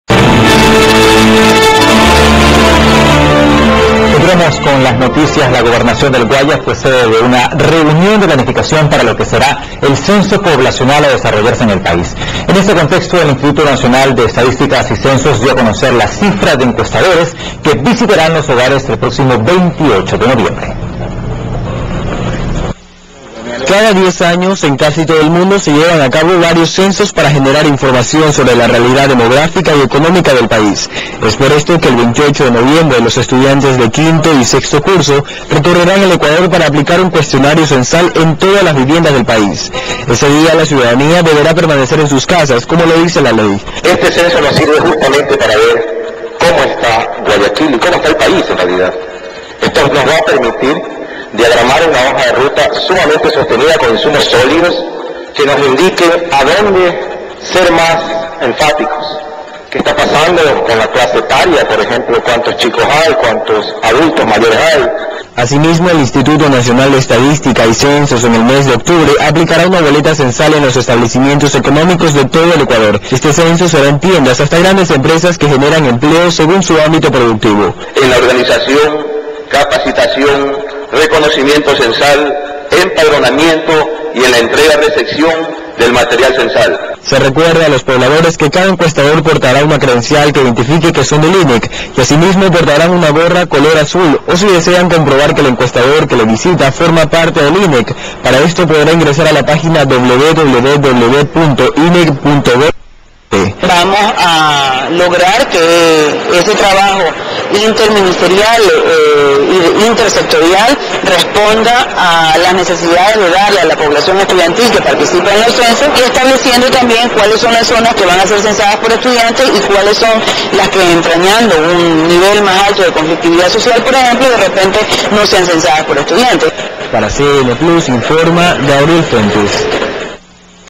Planificación para el Censo de Población.wmv
CABLENOTICIAS.-La Gobernación del Guayas fue la sede de una reunión de planificación para el Censo de Población y Vivienda. En este contexto, el Instituto Nacional de Estadística y Censos dio a conocer el número de empadronadores que visitarán los hogares el próximo 28 de noviembre.
27/09/2010